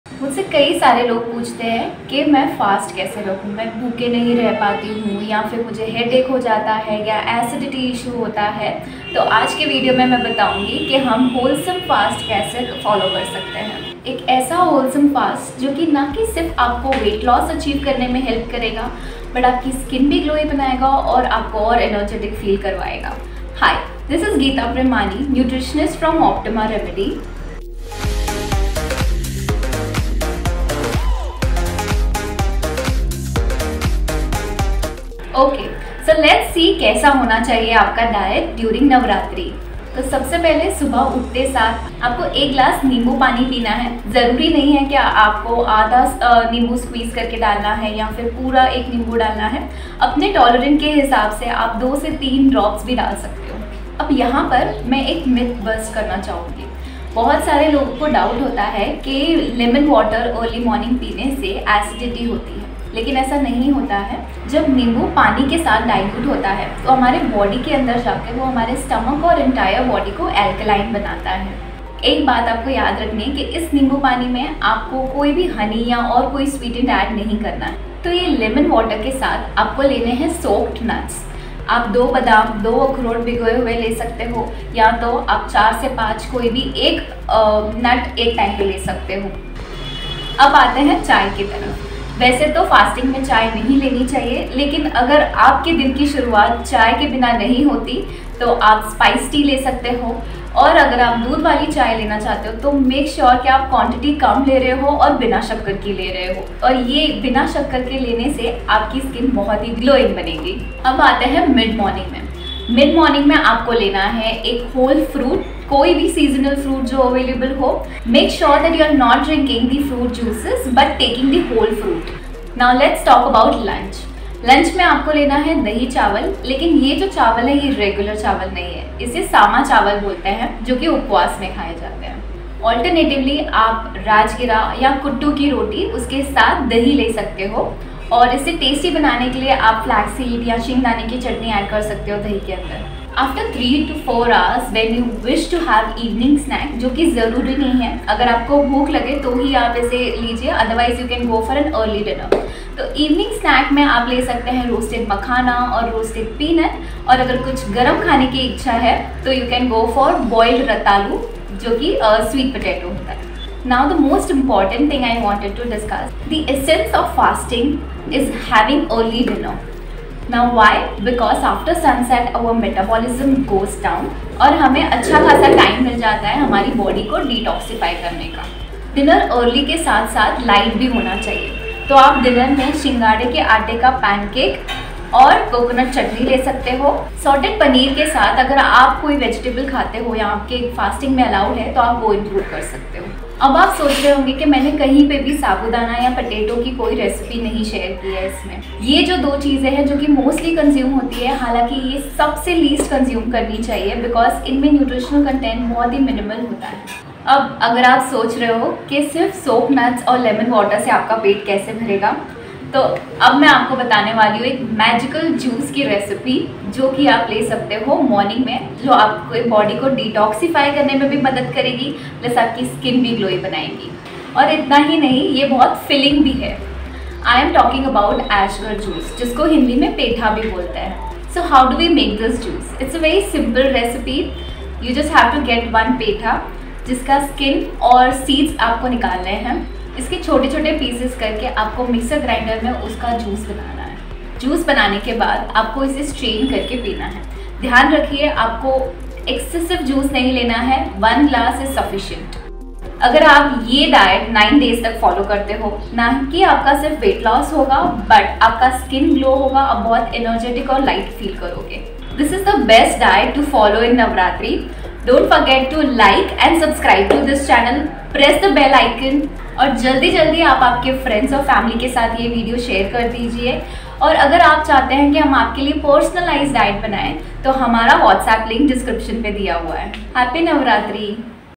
Best Navratri Diet Plan 2022 | Hindi | Weight loss | Geeta Premani | Optima Remedy
Fasting is our Traditional practice which is now observed for health benefits such as intermittentfasting.

In this video we have given the best and a wholesome diet plan to follow this Navratri. This plan will help you detox your body, lose weight, feel energetic, make your skin glow internally. It will also help you improve your mental and physical health.

To get such tips and advice from optima remedy.